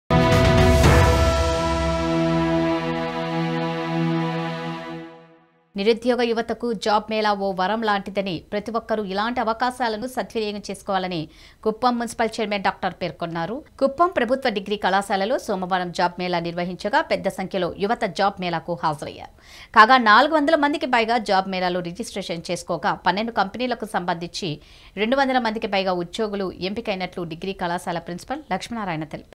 జాబ్ మేళ నిర్వహించగా పెద్దసంఖ్యలో హాజరు 200 మందికి పైగా ఎంపిక
జాబ్ మేళ నిర్వహించగా పెద్దసంఖ్యలో హాజరయ్యారు 200 మందికి పైగా ఉద్యోగులు ఎంపికైనట్లు సమాచరం...

నిరుద్యోగ యువతకు జాబ్ మేళా ఓ వరం లాంటిదని ప్రతి ఒక్కరూ ఇలాంటి అవకాశాలను సద్వినియోగం చేసుకోవాలని కుప్పం మున్సిపల్ చైర్మన్ డాక్టర్ పేర్కొన్నారు కుప్పం ప్రభుత్వ డిగ్రీ కళాశాలలో సోమవారం జాబ్ మేళ నిర్వహించగా పెద్దసంఖ్యలో యువత జాబ్ మేళ కు హాజరయ్యారు కాగా 400 మందికి పైగా జాబ్ మేళాలో రిజిస్ట్రేషన్ చేసుకోగా 12 కంపెనీలకు సంబంధించి 200 మందికి పైగా ఉద్యోగులు ఎంపికైనట్లు డిగ్రీ కళాశాల ప్రిన్సిపాల్ లక్ష్మీనారాయణ తెలిపారు